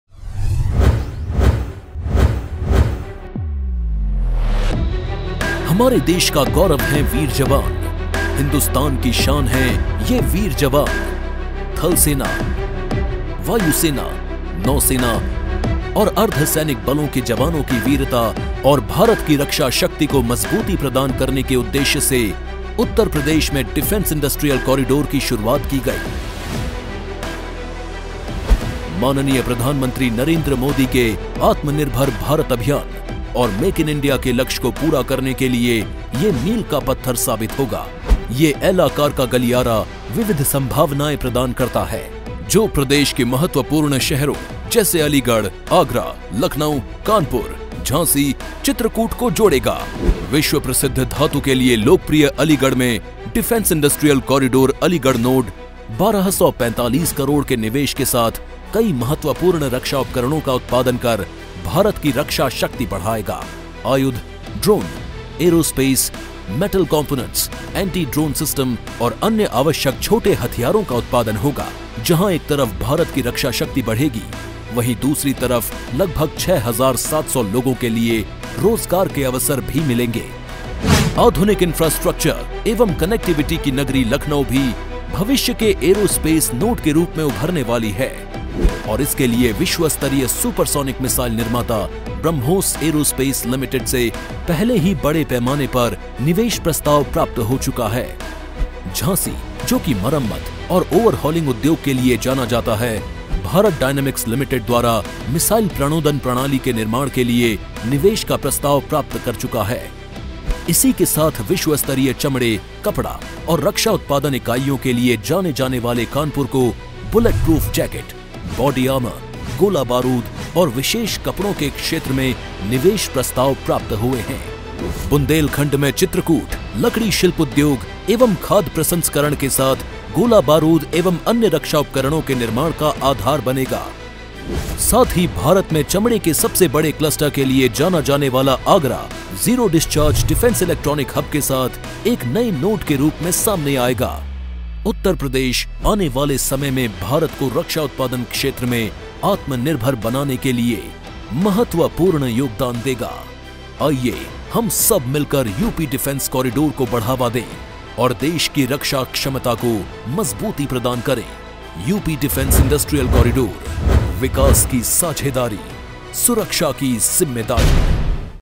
UP Defense Industrial Corridor: Making India self-reliant in defense production
The establishment of a Defense Industrial Corridor in Uttar Pradesh was announced by the Prime Minister while inaugurating the UP Investors Summit in Lucknow on February 21, 2018. A total of 6 nodes – Aligarh, Agra, Kanpur, Chitrakoot, Jhansi and Lucknow – have been planned in the Defense Industrial Corridor. In the Aligarh Node, land allocation process has been completed and land has been allotted to 19 firms, who will invest Rs 1245 crore in the node.

The Defense Industrial Corridor of Uttar Pradesh will help in making the country self-reliant in the field of defense production and promoting 'Make in India'.